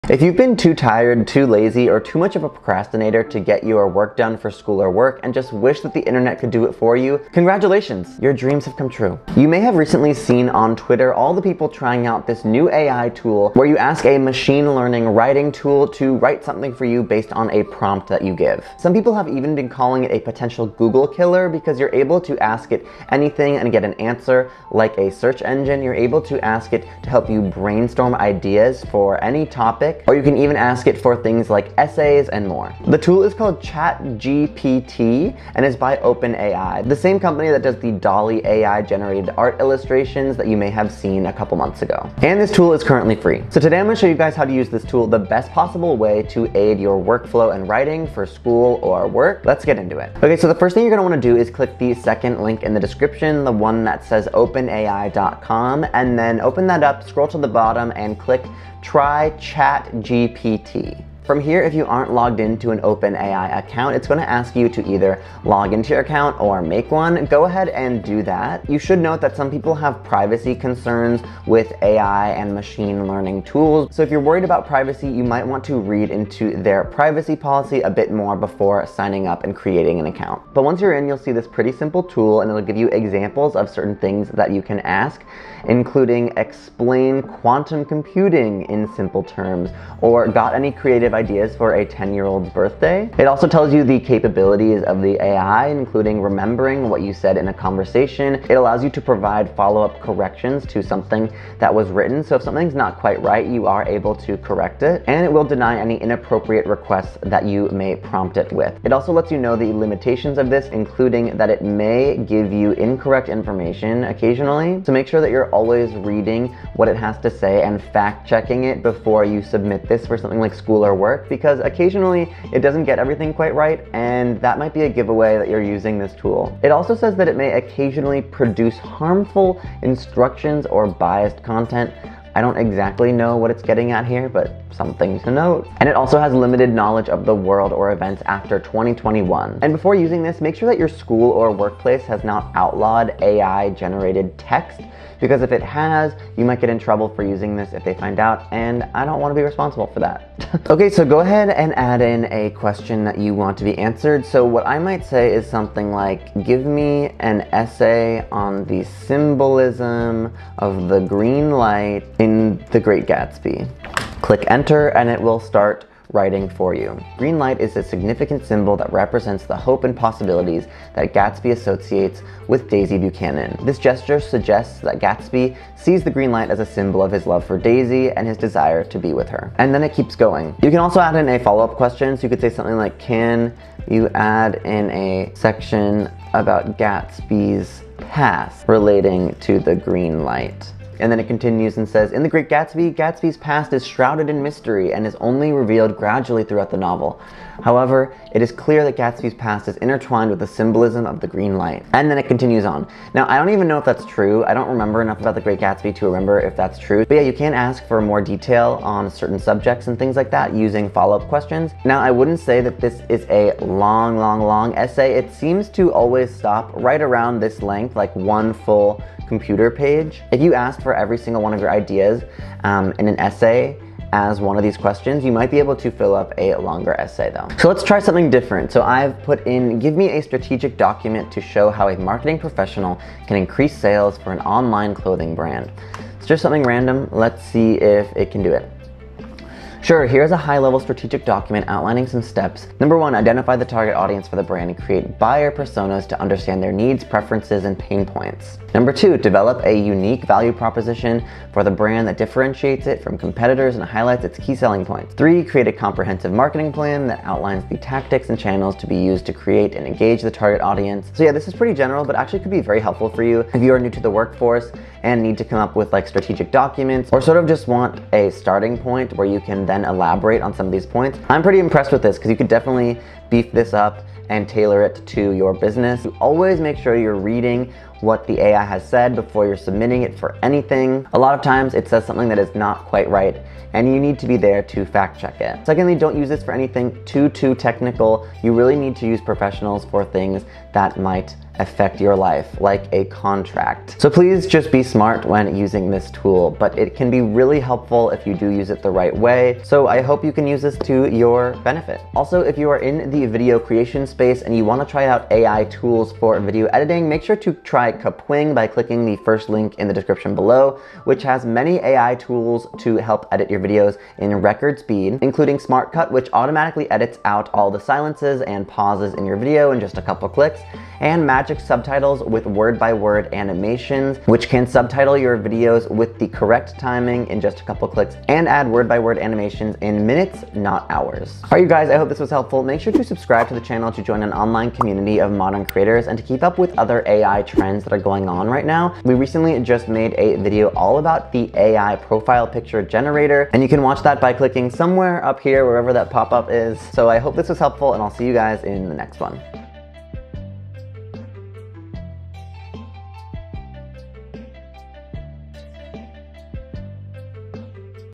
How to Use AI to Write Essays, Projects, Scripts Using ChatGPT OpenAi
AI is becoming more and more powerful, and you are now able to ask AI to write papers and essays, projects, brainstorms, strategic documents, and video scripts for you using a program called ChatGPT, the new machine learning writing tool by OpenAi, the creators of Dall-E illustrations. Today, I share with you how to use it, best practices, and what to be aware of when using this new technology.

Intro to AI generated text 0:00
Privacy, Limitations, and Capabilities 0:50
Using ChatGPT for School (Essays, Follow-ups) 2:31
Using ChatGPT for Work (Strategy documents, Brainstorms, Scripts) 3:48
How to use AI for Video Creation 5:02
Outro 5:32